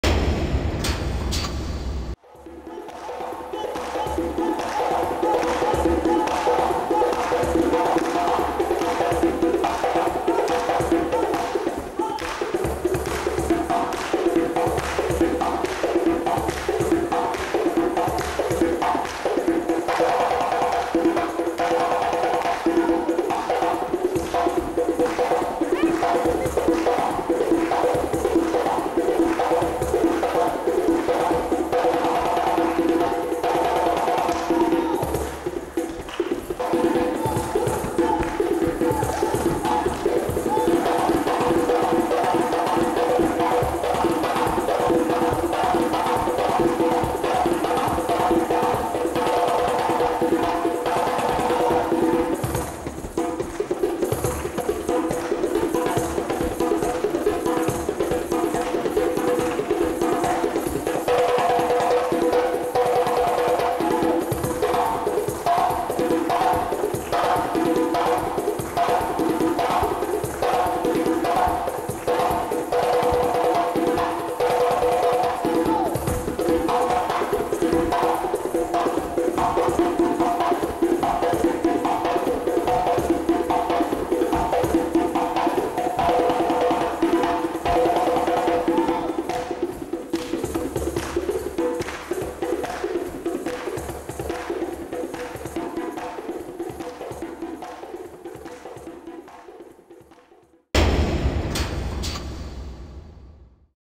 Forum des associations de Rouen de Normandie 1ère Edition le 5 novembre 1994 à Rouen 4/6
Africa Dance Soirée Dansante au Forum des associations de Rouen de Normandie 1ère Edition le 5 novembre 1994 à Rouen organisé par l'association ASPRA